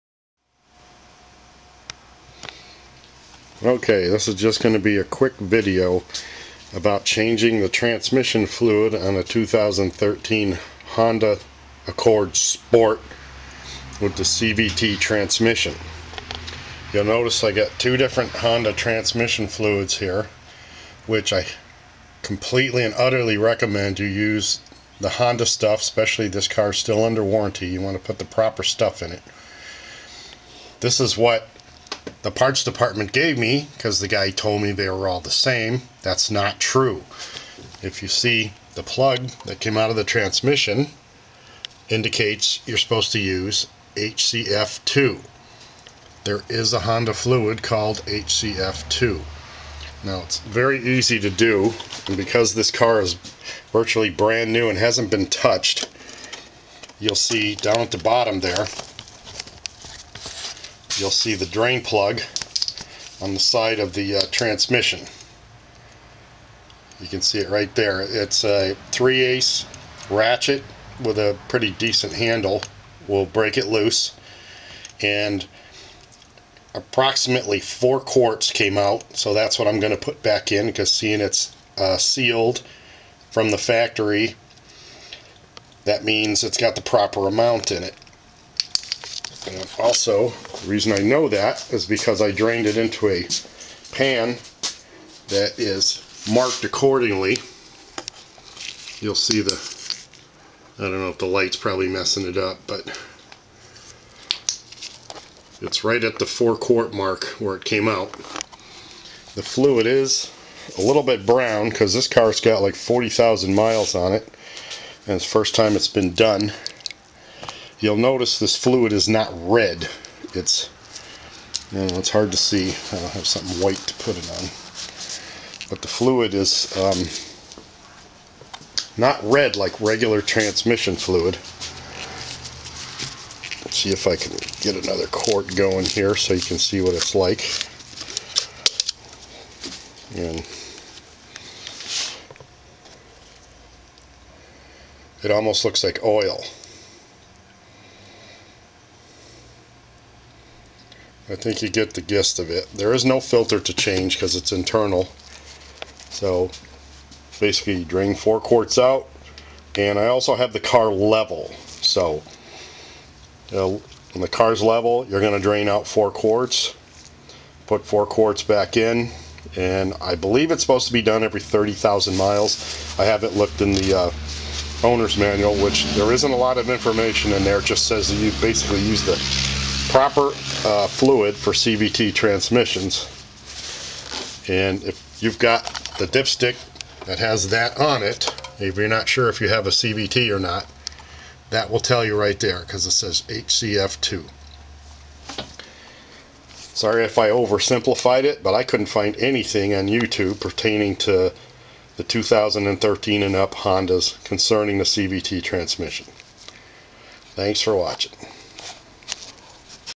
2013 Honda Accord Sport CVT HCF-2 transmission fluid change.
Just a quick VID on doing a transmission fluid change on a Honda equipped with a CVT transmission. One thing I didn't mention was the drain plug has a magnet and should be cleaned prior to reinstalling it. Remember! HCF-2 fluid ONLY in a CVT. Page 508 in the owners manual talks a little about using the proper fluid. In case there is any confusion, that plug with the yellow tag on it came out of the hole the funnel is now in. I drove the car after, runs and drives just fine. The fluid is $11.00 a quart at the dealer.